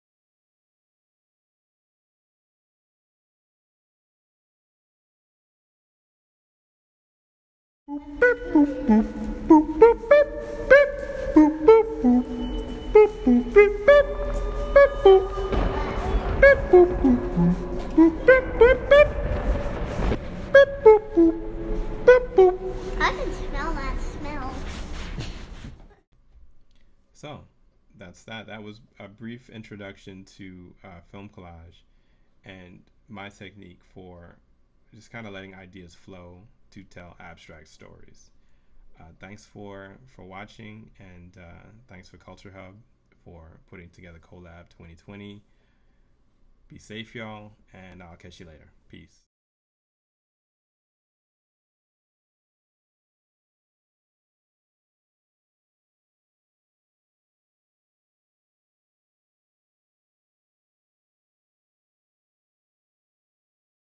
CoLab I Creative Resilience & Resourcefulness with Film Collage | Chameleon Dreams
Creative Resilience & Resourcefulness with Film Collage with Ali Santana

Learn the basics of video editing and tips and tricks on how to be resourceful and creative at home. Multi-disciplinary artist and educator Ali Santana will demonstrate his method of translating personal experience and emotion into abstract video art. Ali will explain his work, process, and appreciation for abstract storytelling using found footage and free software.